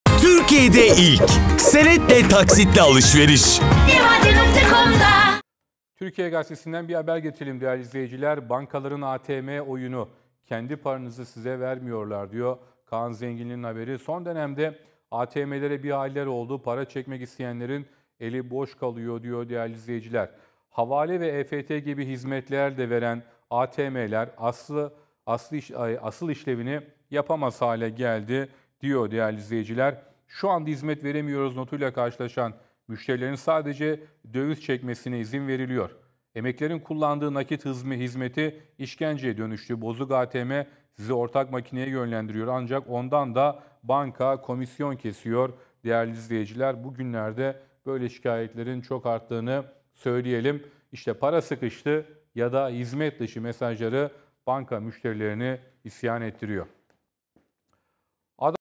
Bankaların ATM Oyunu! Kendi Paranızı Size Vermiyor! - TGRT Haber
ATM şikâyetleri her geçen gün artıyor. Uzmanlar yaşanan sürecin bilinçli olduğunu iddia ederek, bankaların bu durumu lehine çevirdiklerini belirtiyor. Tüketici dernekleri ‘Aynı ATM iki gün para vermezse suç’ diyor.
Haberin detaylarını Gazeteci Cem Küçük TGRT Haber canlı yayınında aktardı.